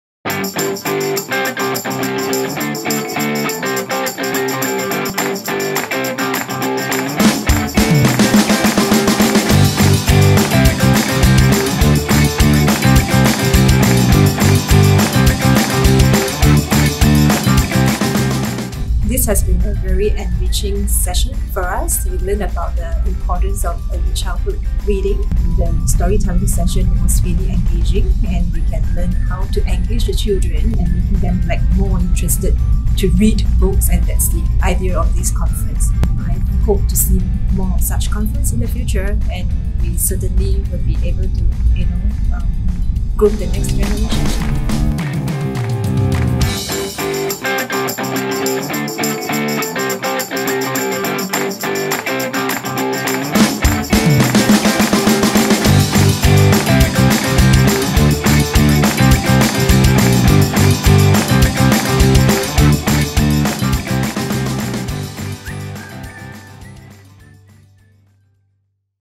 whatwenton during ECLC2019
Here's snippets of what happened during Early Childhood Literacy Conference (ECLC) 2019, held on 16 November 2019 at Resorts World Sentosa. Hear what our EC professionals, parents, partners and children have to say about this year’s ECLC!